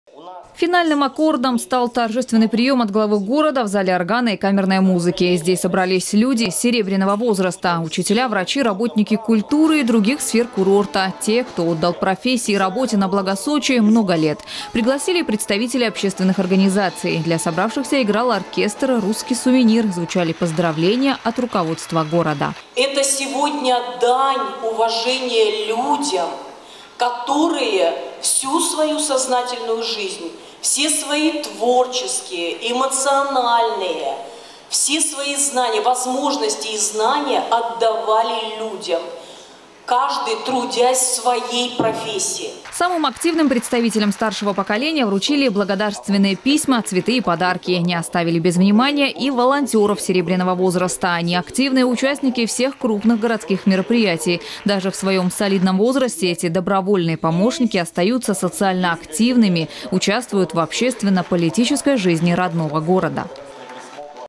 В Сочи чествовали пожилых людей. Новости Сочи Эфкате
В Сочи завершилась череда мероприятий, посвящённых Международному Дню пожилого человека. Эту дату отмечают 1 октября и с этого дня во всех районах курорта проходили праздничные мероприятия и концерты.
Сочи. Новости.
Телеканал Эфкате.